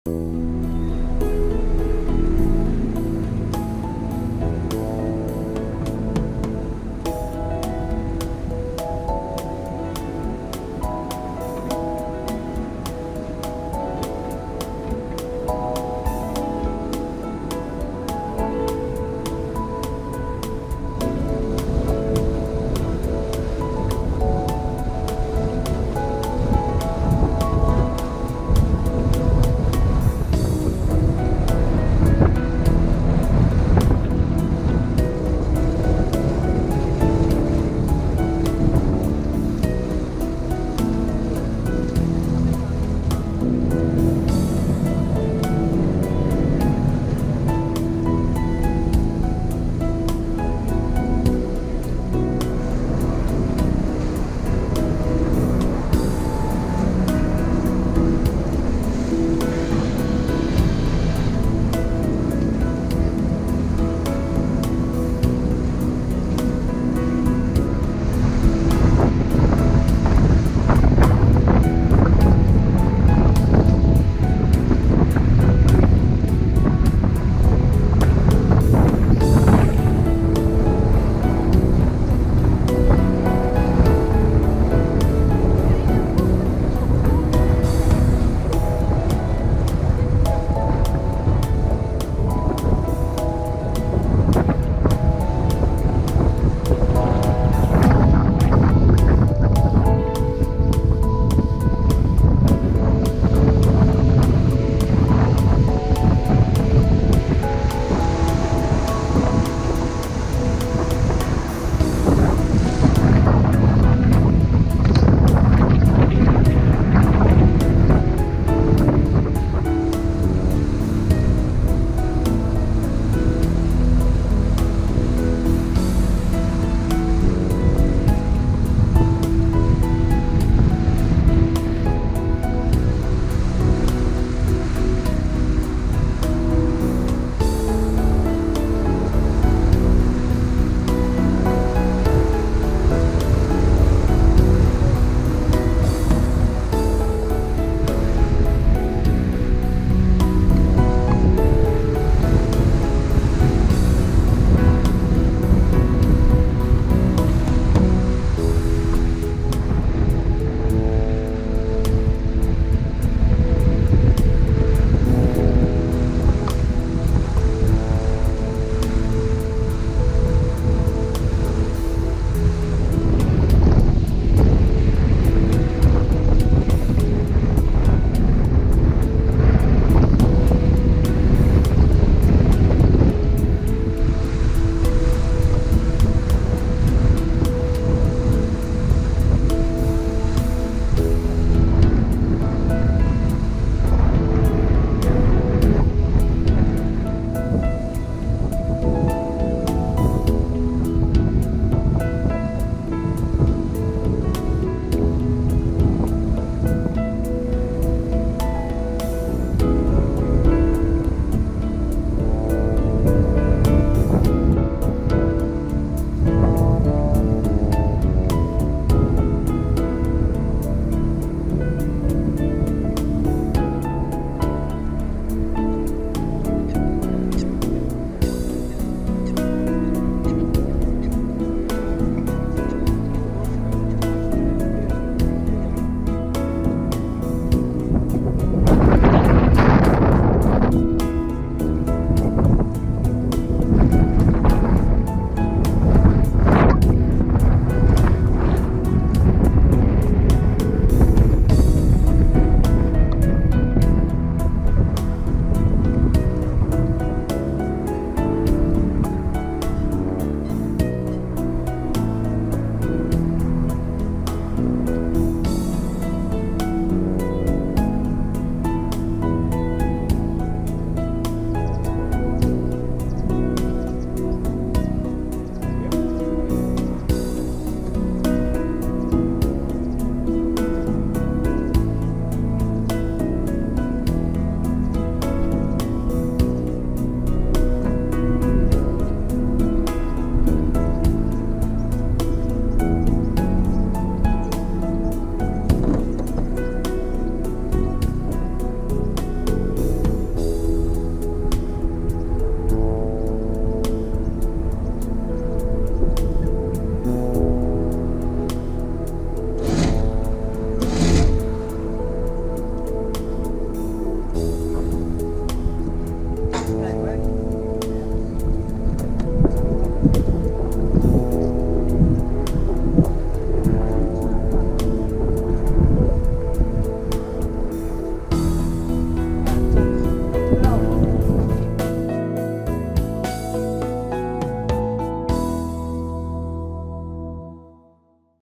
Van Zwolle naar Hattem
Met de boot van Zwolle naar Hattem.